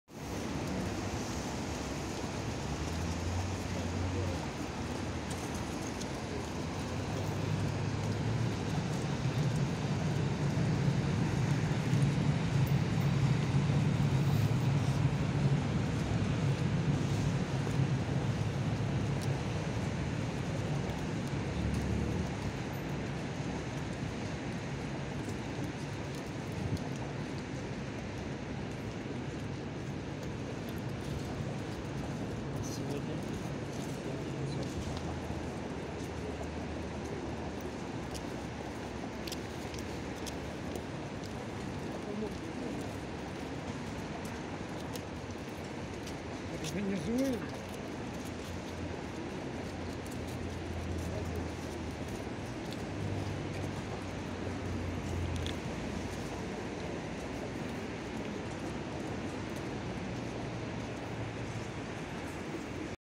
На Грушки ? грузовый поезд под вл80 с Киев-Волынского по соединительной ветке Борщаговка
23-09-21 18ч На Грушки(Киев Октябрский)? грузовый поезд под вл80 с Киев-Волынского по соединительной ветке Борщаговка